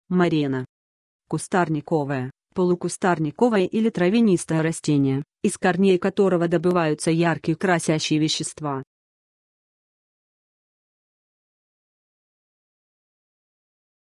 МАРЕНА - что это такое? значение и описание
Хочешь знать что значит МАРЕНА? тогда слушай до конца. n n -ы, ж. Кустарниковое, полукустарниковое или травянистое растение, из корней к-рого добываются яркие красящие вещества. || прил. мареновый, -ая, -ое и маренный, -ая, -ое. Семейство мареновых (сущ.). Мареновый цвет (ярко-красный). n n Больше слов